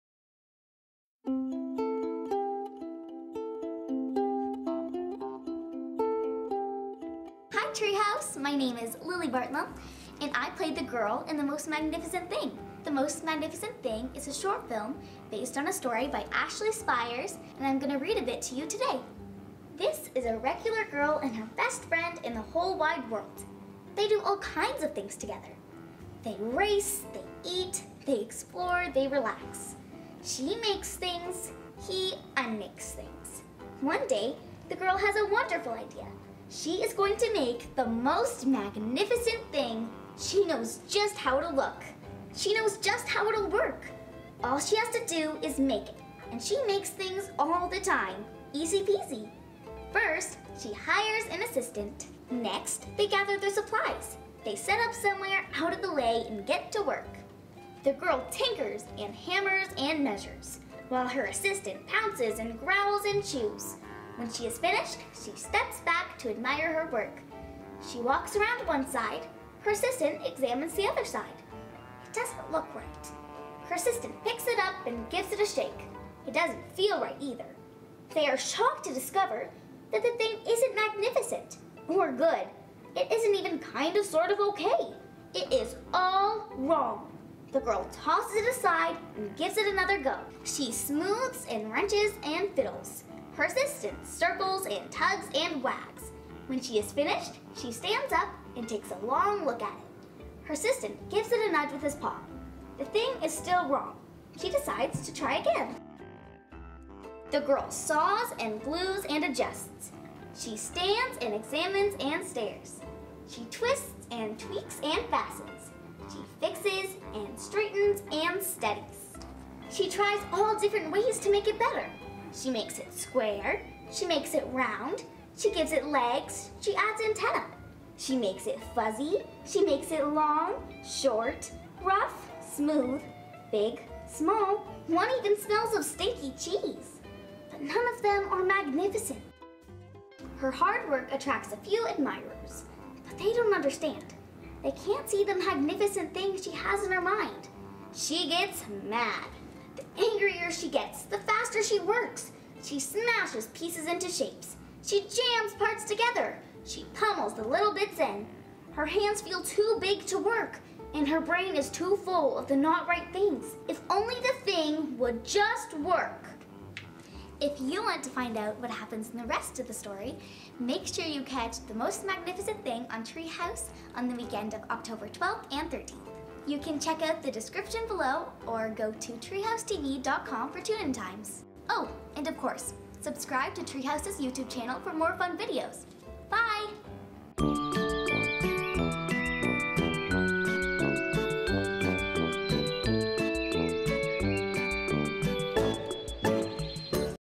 Story Time! w/ Lilly Bartlam | "The Most Magnificent Thing" by Ashley Spires | Treehouse
The short animated film "The Most Magnificent Thing" is coming to Treehouse & star Lilly Bartlam stopped by Treehouse to read a snippet from the storybook by Ashley Spires that inspired the film.

The Most Magnificent Thing premieres October 12th at 9:45am, and continues throughout the Most Magnificent Weekend on Treehouse. for more magnificent times to catch The Most Magnificent Thing!